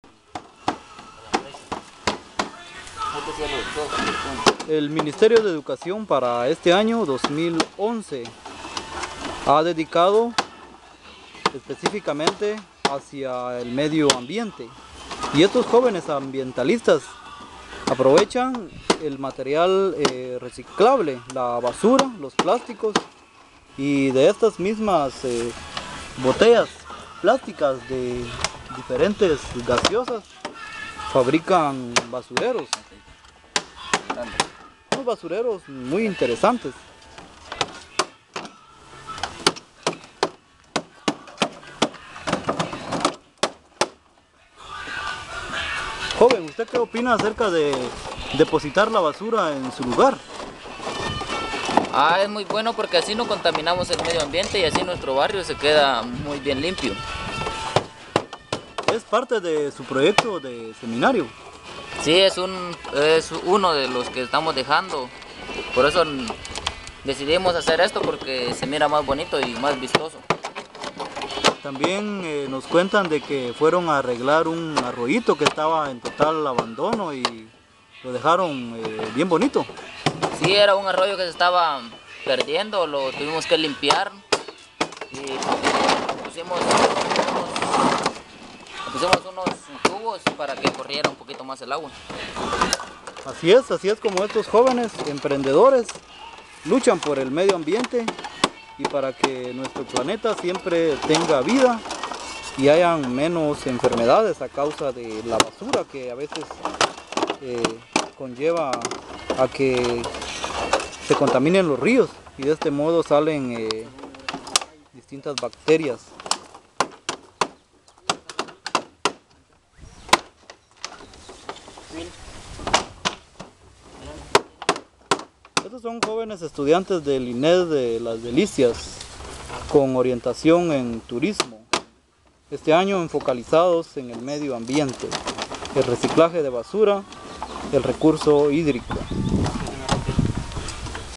Basureros de botellas plásticas
Jóvenes estudiantes del INED-LAS DELICIAS, recolectan botellas plásticas y fabrican basureros con las mismas, como parte de su proyecto de seminario.